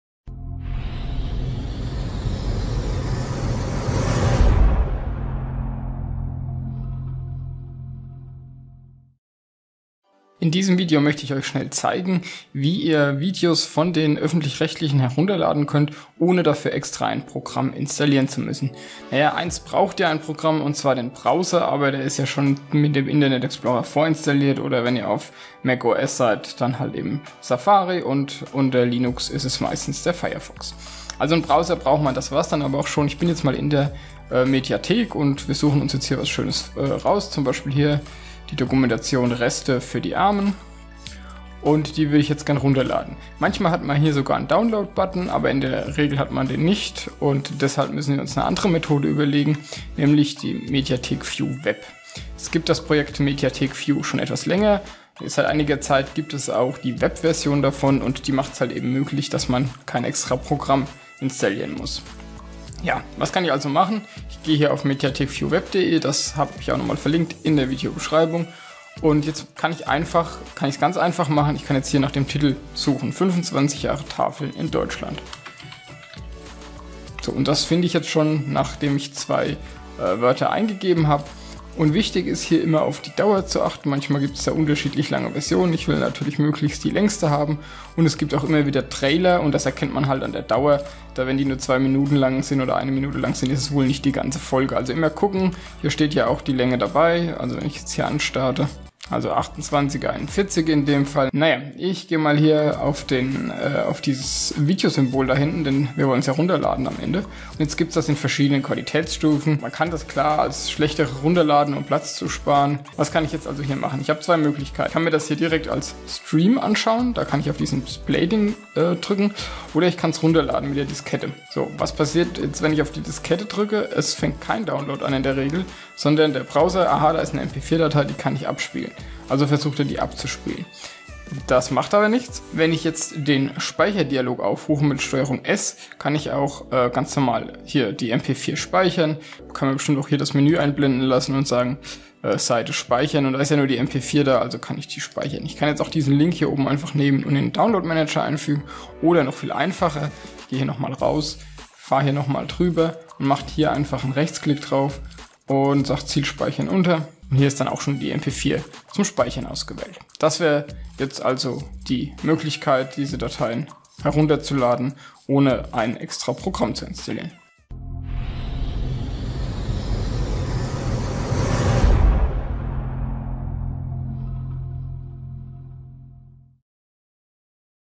Anleitung: Video Download aus der Mediathek ohne (zusätzliches) Programm
WERBUNG Mediathek auf dem TV
So kann man Videos aus den Mediatheken der öffentlich Rechtlichen mit nur wenigen Klicks herunterladen. Dank geht an das Projekt Mediathekviewweb!
Für Android gibt es in F-Droid die App ZAPP
Es geht hier nur um die öffentlichen rechtlichen! Es geht nicht um private Sender wie sat.1 pro 7 vox usw.